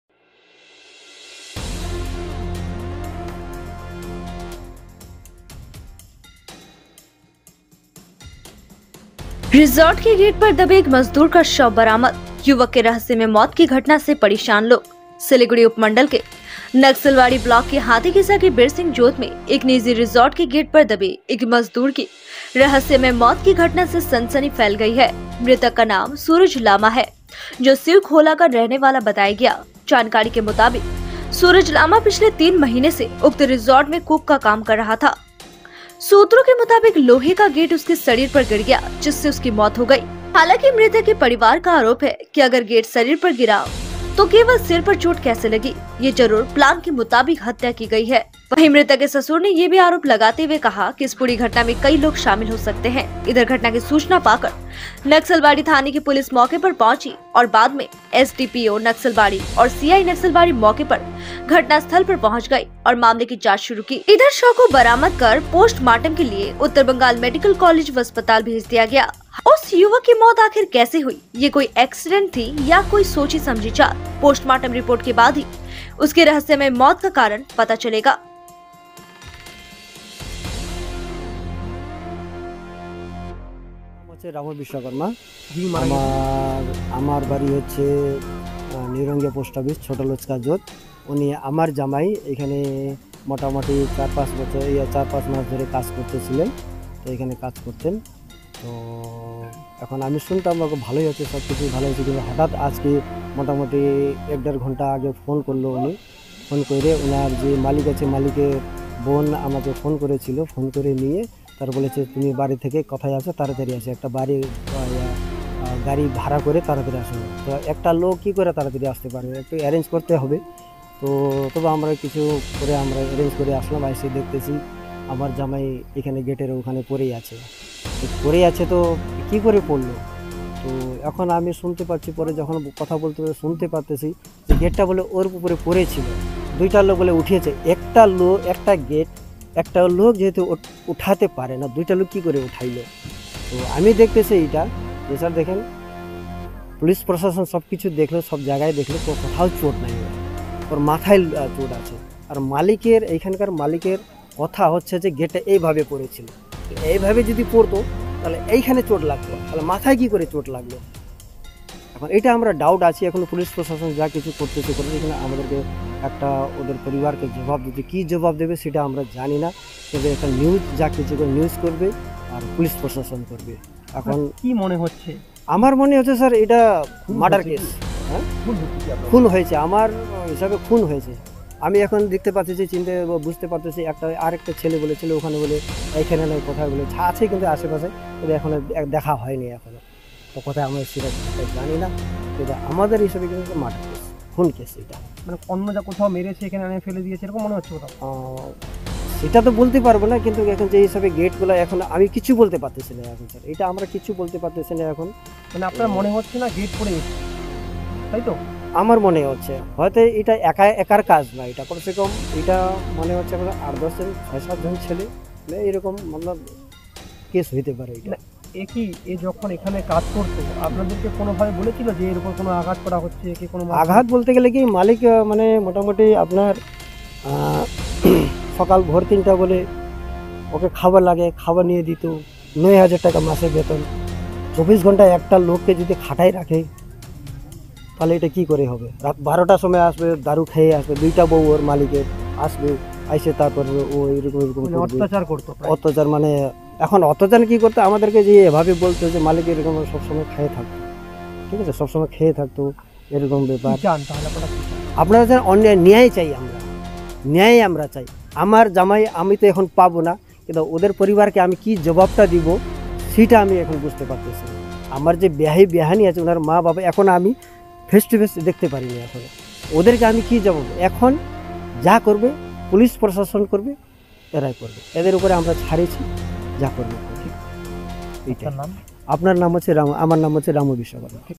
Siliguri : The body of a laborer was found buried under the gate of the resort
Siliguri : The body of a laborer was found buried under the gate of the resort; People are worried about the mysterious death of the young man